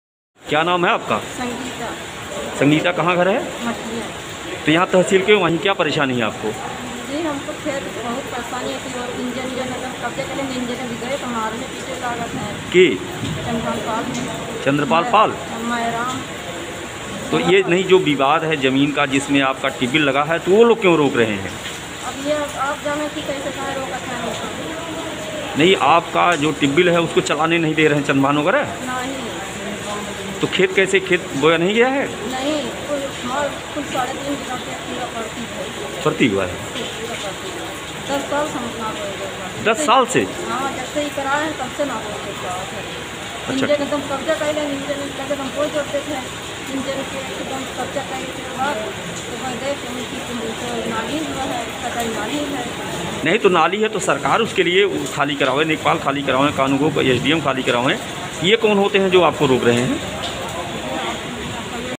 Hindmorcha Video Ambedkarnagar Jamin Kabja
दबंग बिपक्षियो के आगे महिला 10 वर्षो से अपने ही जमीन पर कब्जा दखल से महरूम...देखें Video